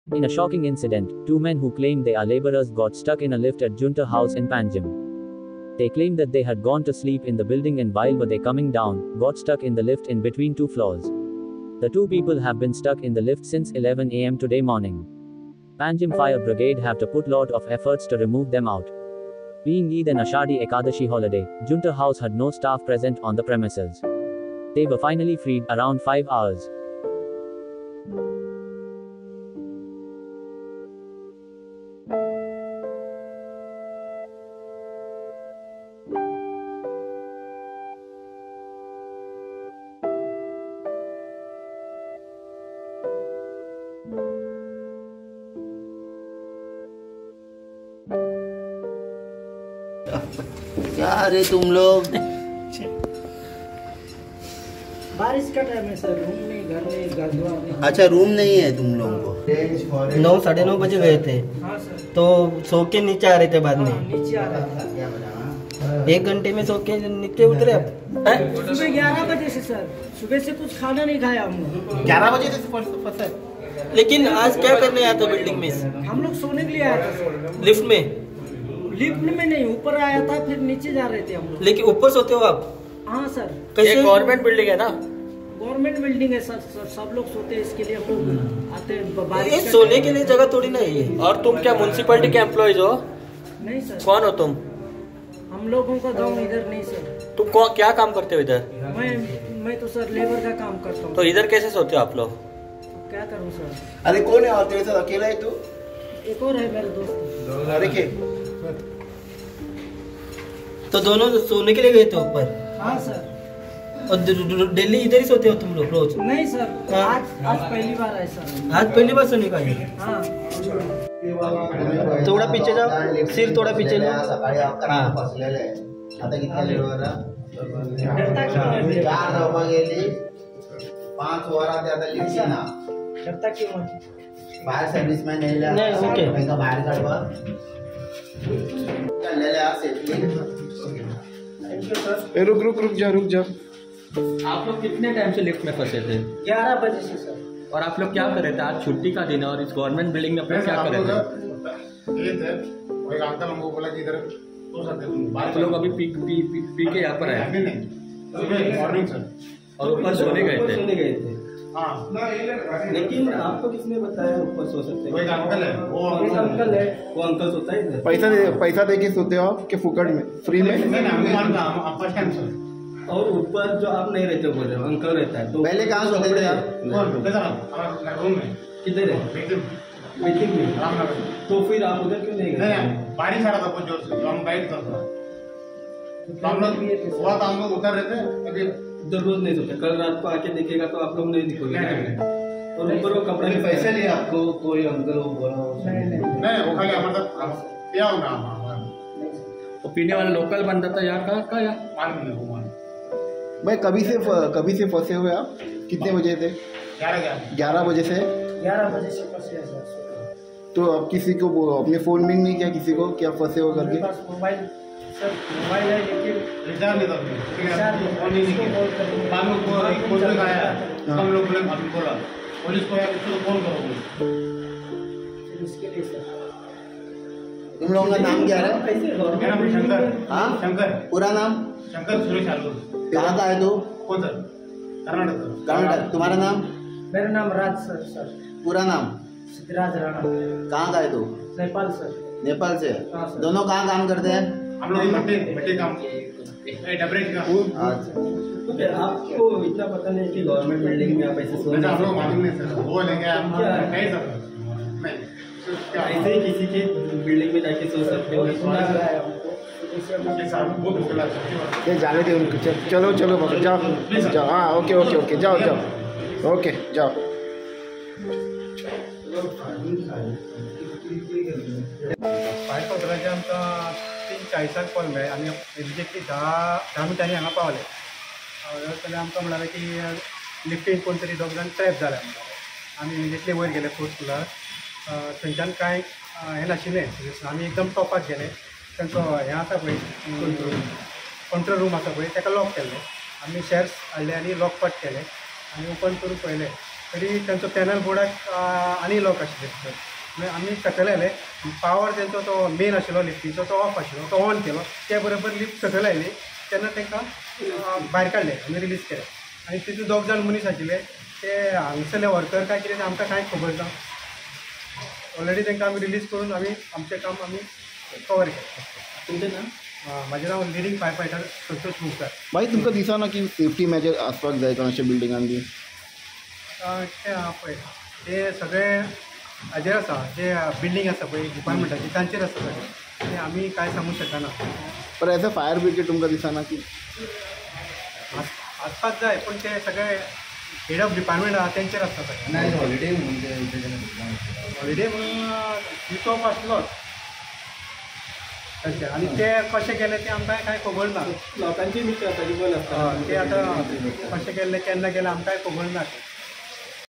Two Labour got stuck in the lift for five hours..
In a shocking incident, two men who claim they are labourer's got stuck in a lift at Junta House in Panjim. They claim that they had gone to sleep in the building and while were they coming down, got stuck in the lift in between two floors. The two people have been stuck in the lift since 11 am today morning. Panjim fire brigade have to put lot of efforts to remove them out. Being Eid and Ashadi Ekadashi holiday, Jhunta house had no staff present on the premises. They were finally freed around 5 hours.